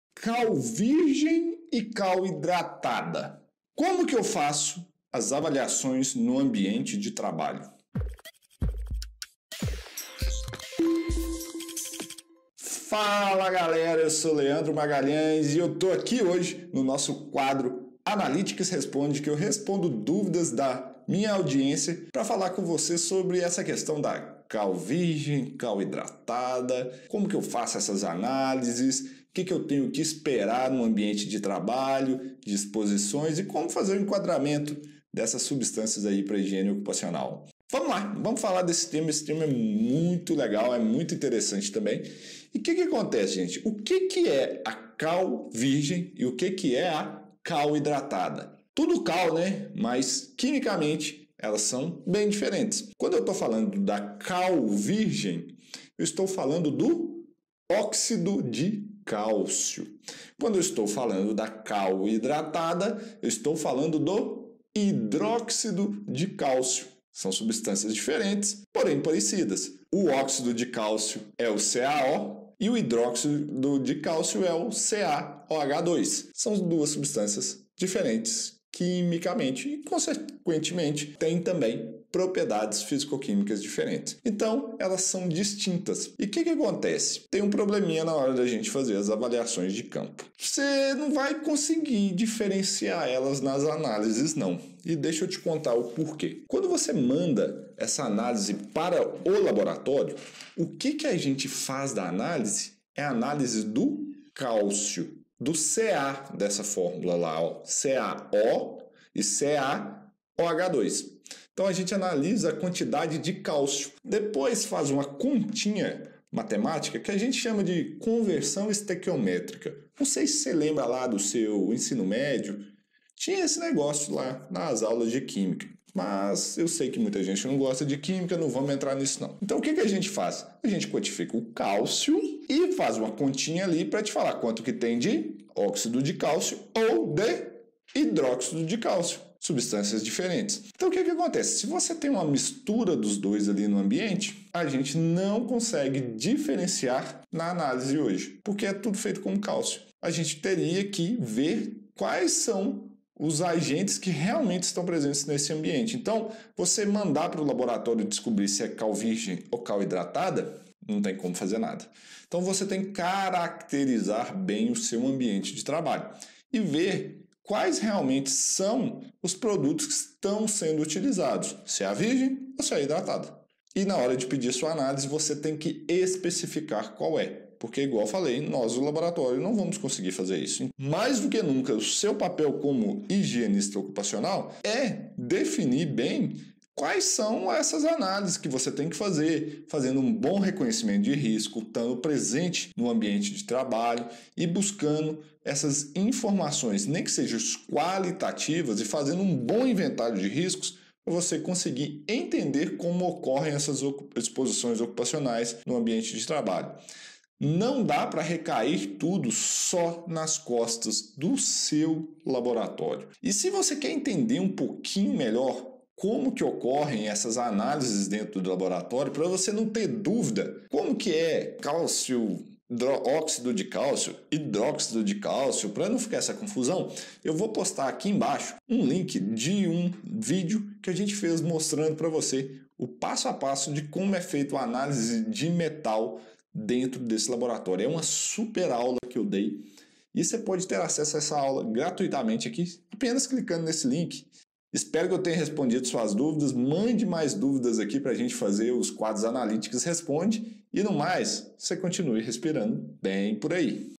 Cal virgem ou Cal hidratada como avaliar? | Analytics Responde #24 | Higiene Ocupacional
Cal virgem ou Cal hidratada?

Apesar de serem parecidas, tem propriedades diferentes.

Neste episódio, eu irei mostrar e comparar as duas para você não ter mais dúvidas!

Acompanhe o vídeo e continue respirando bem por ai!

Obrigado.